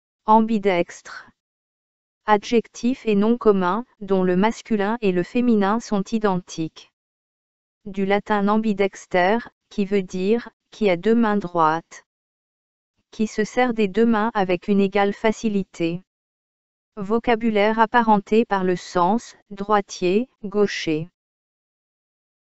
Ambidextre | Dictionnaire français audio
Définition du terme Ambidextre.
Adjectif et Nom commun, dont le masculin et le féminin sont identiques.
Du latin ambidexter, (qui veut dire : « qui a deux mains droites »).
Qui se sert des deux mains avec une égale facilité.
Vocabulaire apparenté par le sens : droitier, gaucher.